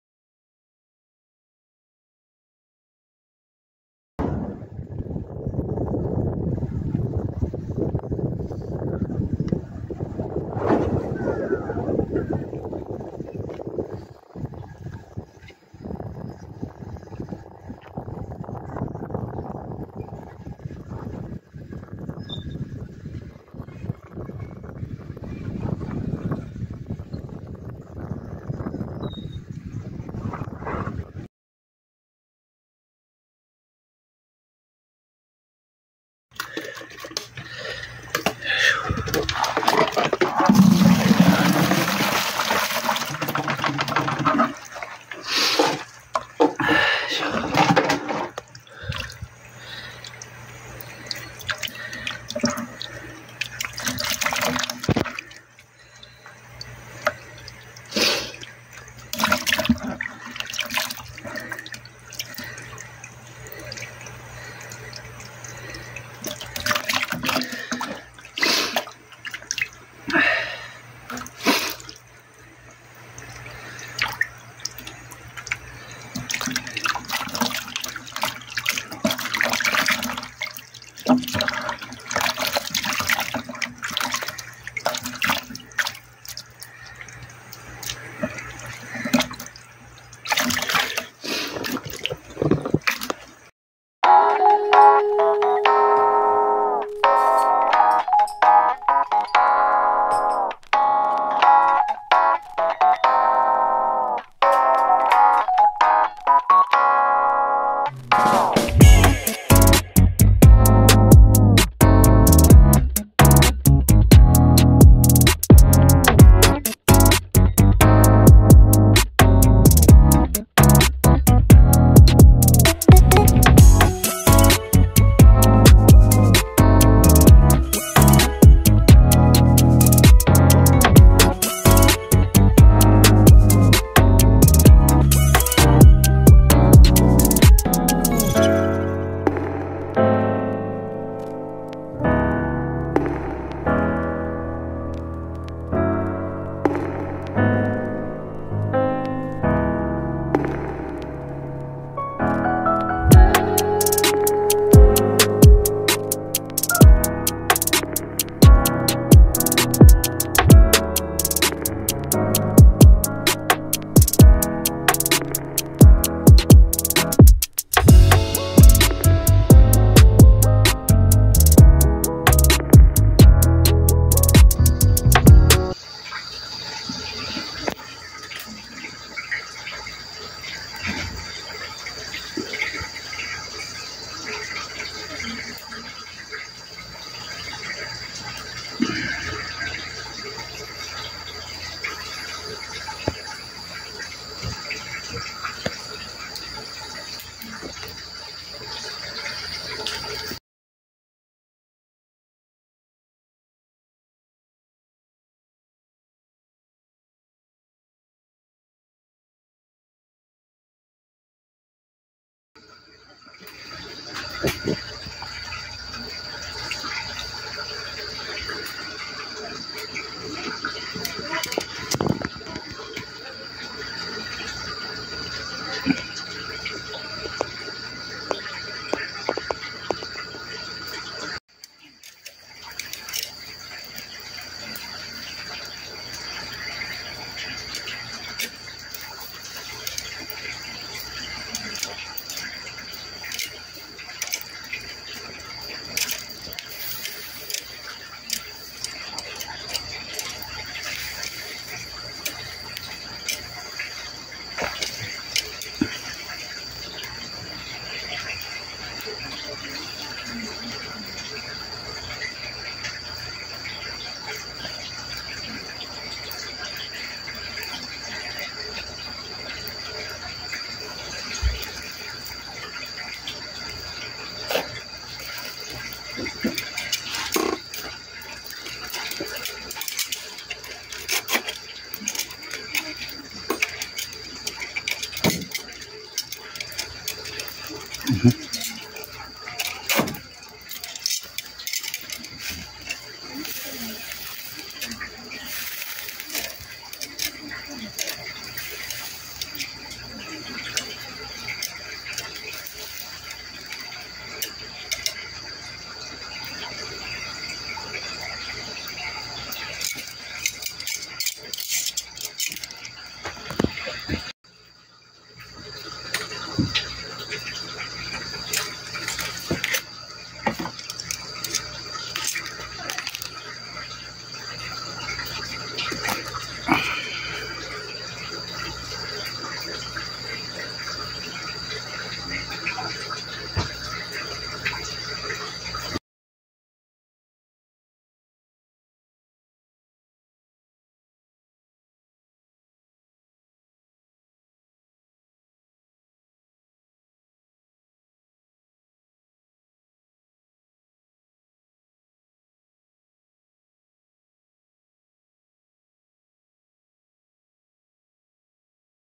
海・川・山を満喫！！
自然に恵まれた環境で海・川・山を満喫！そんな最近の出来事のまとめ動画です。